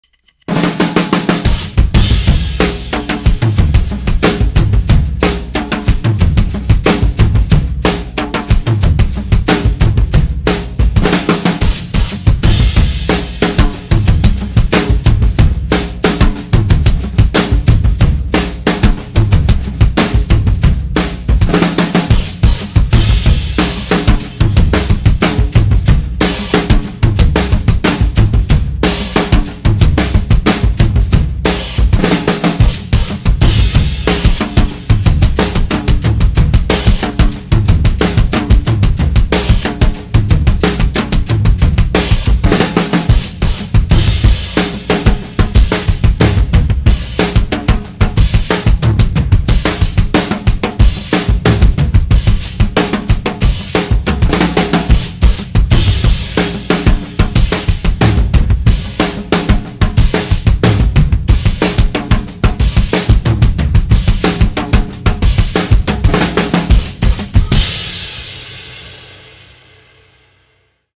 DrummerPlusDrummer: Brendan Buckley & Tucker Rule (4/12/21)
🥁 DrummerPlusDrummer 🥁 This week’s installment of @drummerplusdrummer features @tuckerrule .  I was initially turned on to Tucker’s drumming through our mutual connection with the great @8gband from @latenightseth (meyers) and producer @erx11 .  However, Tucker is most widely known for his powerful drumming with the punk group @thursdayband .  ⚡️ Thanks for jamming with me Tucker!  @brendanbuckley
(🤘New Jersey🤘)